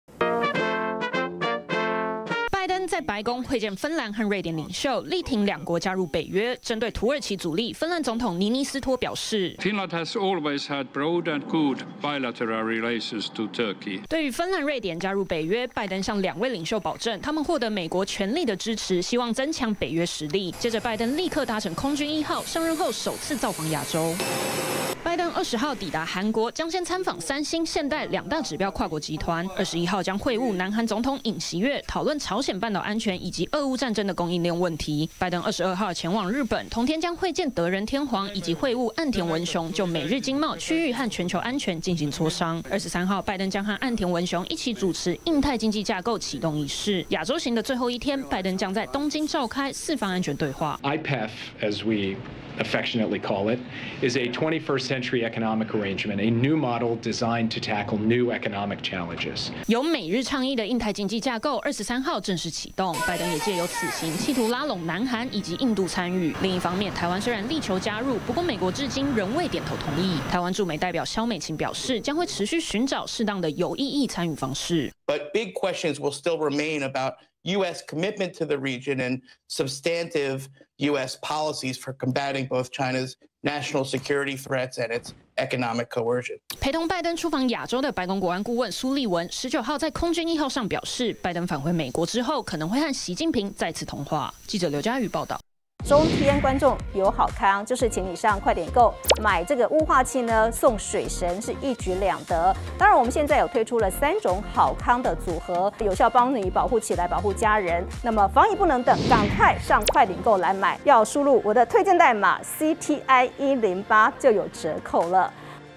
【每日必看】拜登首訪亞洲會日韓領袖 5/23正式宣布成立IPEF@中天新聞CtiNews 20220520
美國總統拜登，美東時間5/19號，會晤芬蘭瑞典領袖，對兩國申請加入北約，表示大力支持。隨後立刻搭乘空軍一號，開啟上任後第一次的亞洲行。拜登將先會晤新上任的、南韓總統尹錫悅。之後將飛往日本，與首相岸田文雄會談，並一起在23號時，主持印太經濟架構啟動儀式。外界也預估，拜登會藉由此行，拉攏南韓以及印度，加入美國主導的印太經濟架構。